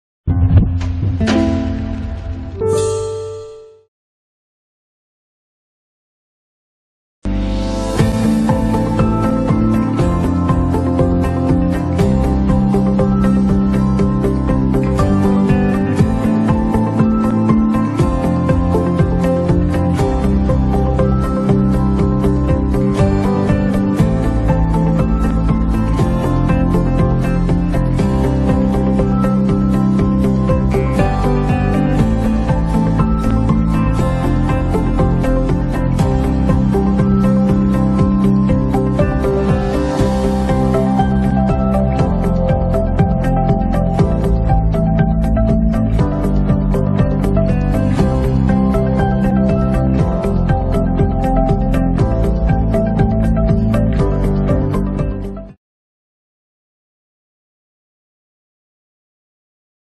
MSBC Richpeace Quilting Machine
World's First & Only 1500 RPM High Speed Quilting Machine.